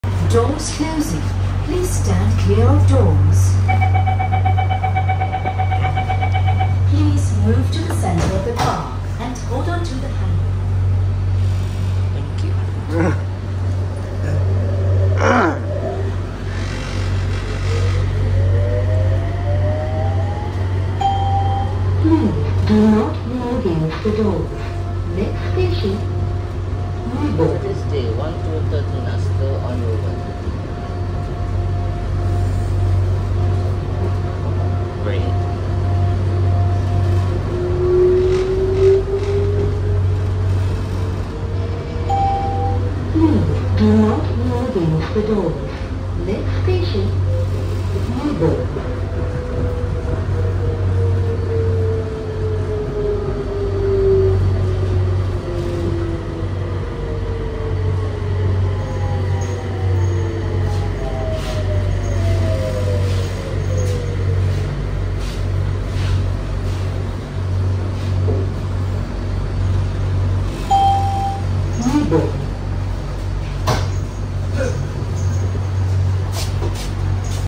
MRTravels on the Punggol LRT: C810 V01 from Samudera to Nibong
This video was filmed on 3 November 2024.

C810 V01 PGLRT West ACW